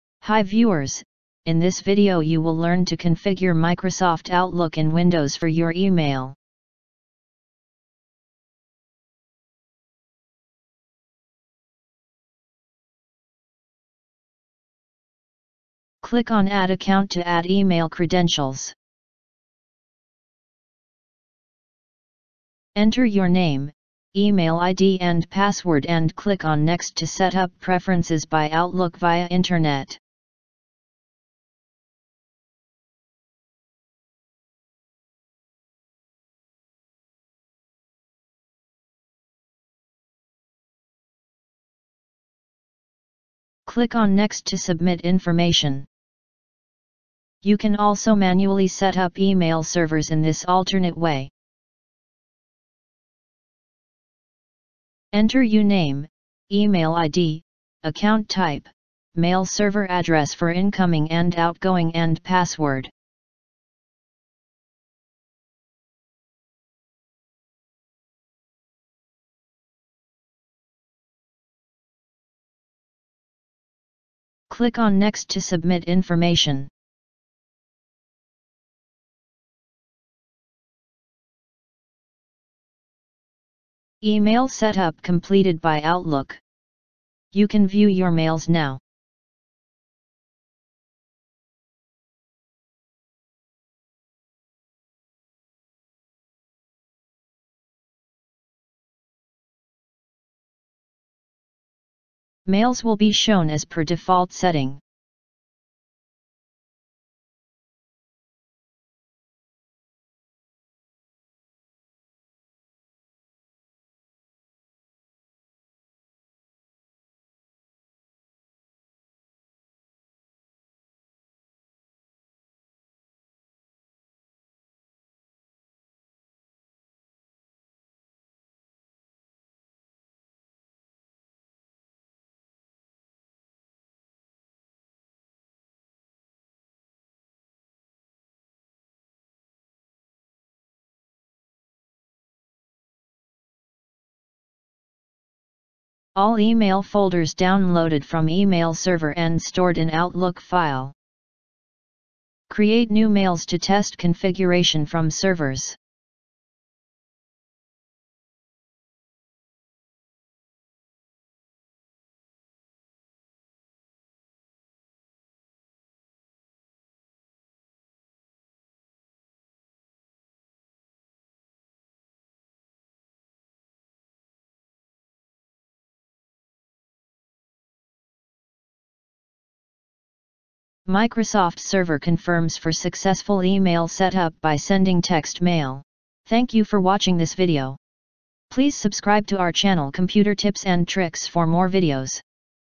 How to Configure outlook for Email | Setup outlook for First use | Microsoft Outlook
The steps in this video describe how to set up your email account using Outlook 2016, Outlook 2013, or Outlook 2010 on your PC.

Outlook comes with Microsoft Office for Windows and can be used alone to manage mail via IMAP or POP. It provides an integrated email and calendar experience.